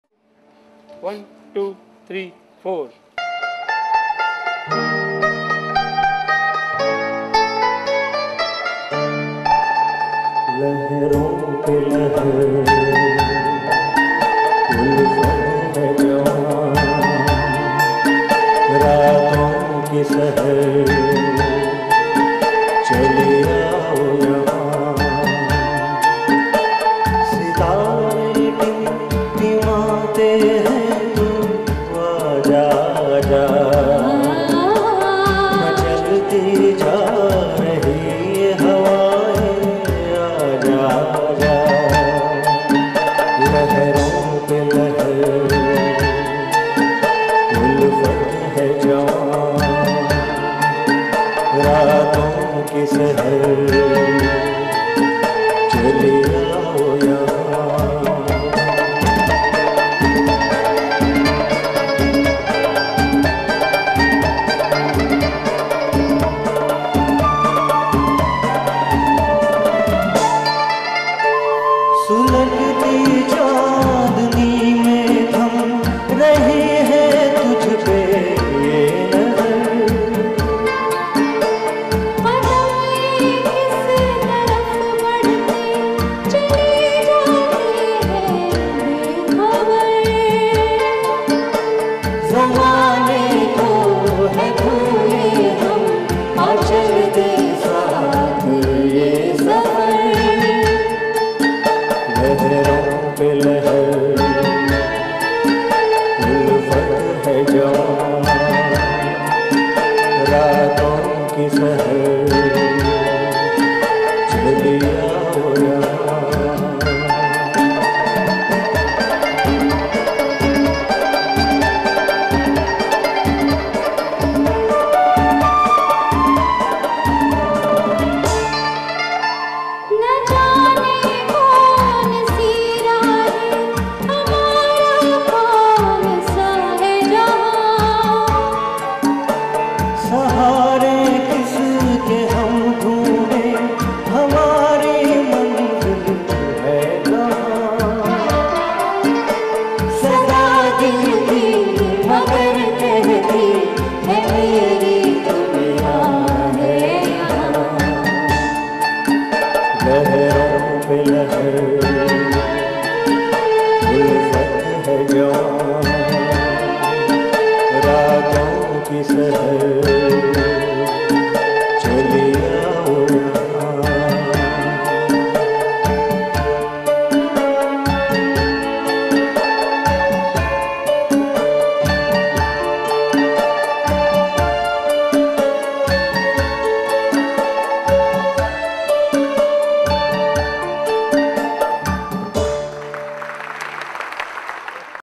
0021....APARA MUSICS PAYS A HUMBLE TRIBUTE TO THE GREAT HEMANT KUMAR
APARA MUSICS ARRANGED A LIVE SHOW TO PAY A MELODIOUS TRIBUTE TO THE GREAT...LEGEND..MUSIC DIRECTOR & SINGER OF OUR GOLDEN ERA...AT RAVINDRA NATAY MANDIR MUMBAI...SHOW NAMED TUMHARA INTZAR HAY PART 1...PLZ. ENJOY A BEAUTIFUL SONG FROM THAT...IF U LIKE SHARE TO OTHERS..HOPE SO...